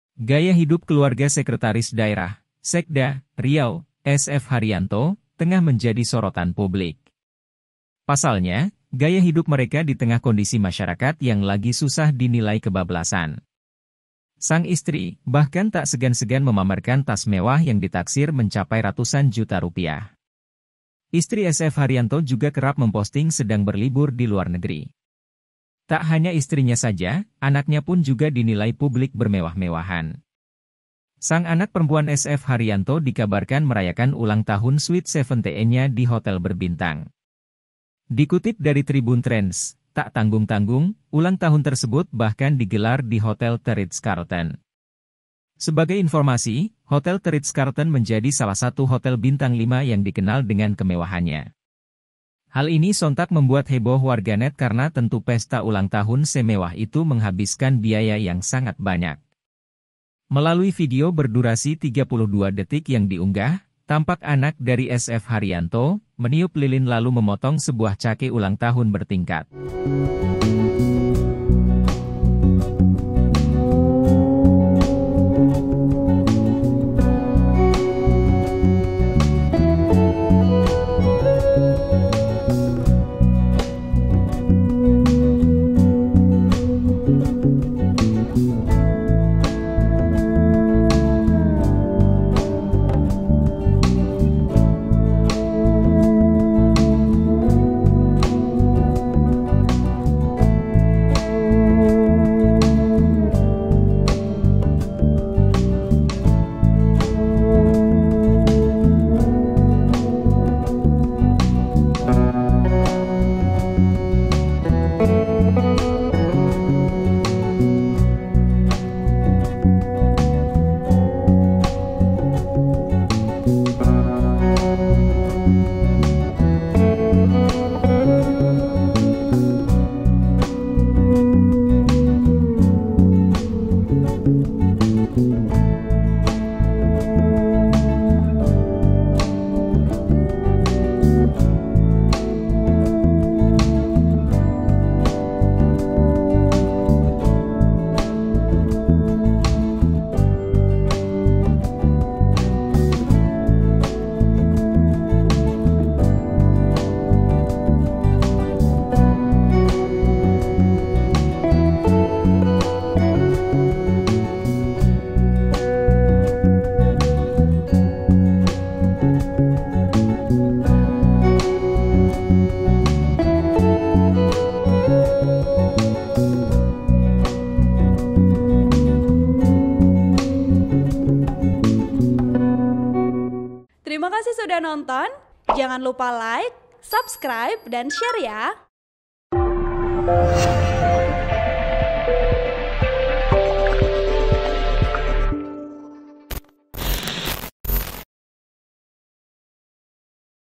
Sekda Riau Bantah Anaknya Rayakan Ultah ke 17 Tahun di Hotel Mewah Bintang Lima Sebut Hanya di Toko
TRIBUN-VIDEO.COM, RIAU - Gaya hidup keluarga sekretaris daerah (sekda) Riau, SF Hariyanto, tengah menjadi sorotan publik.

Pasalnya, gaya hidup mereka di tengah kondisi masyarakat yang lagi susah dinilai kebablasan.

Sang istri, bahkan tak segan-segan memamerkan tas mewah yang ditaksir mencapai ratusan juta rupiah.

Istri SF Hariyanto juga kerap memposting sedang berlibur di luar negeri.

Tak hanya istrinya saja, anaknya pun juga dinilai publik bermewah-mewahan.

Sang anak perempuan SF Hariyanto dikabarkan merayakan ulang tahun sweet seventeen-nya di hotel berbintang.

Dikutip dari TribunTrends, Tak tanggung-tanggung, ulang tahun tersebut bahkan digelar di Hotel The Ritz Carlton.

"Selamat pagi Pak SF Hariyanto, Sekda Riau, apakah video pesta di Ritz Carlton ini adalah pesta puteri Bapak? Jika iya apakah hotelnya Ritz Carlton KW dan pestanya cuma pesta imajinasi?" Tulis akun Twitter @PartaiSocmed dalam cuitannya yang diunggah Senin (20/3/2023).

Sebagai informasi, Hotel The Ritz Carlton menjadi salah satu hotel bintang 5 yang dikenal dengan kemewahannya.

Hal ini sontak membuat heboh warganet karena tentu pesta ulang tahun se mewah itu menghabiskan biaya yang sangat banyak.

Melalui video berdurasi 32 detik yang diunggah, tampak anak dari SF Hariyanto, meniup lilin lalu memotong sebuah cake ulang tahun bertingkat.

Artikel ini telah tayang di TribunJakarta.com dengan judul Dalih Sekda Riau Bantah Anaknya Rayakan Ultah ke-17 di Hotel Mewah Bintang Lima: Cuma di Toko,
Video Production: Tri Susilo Mardhani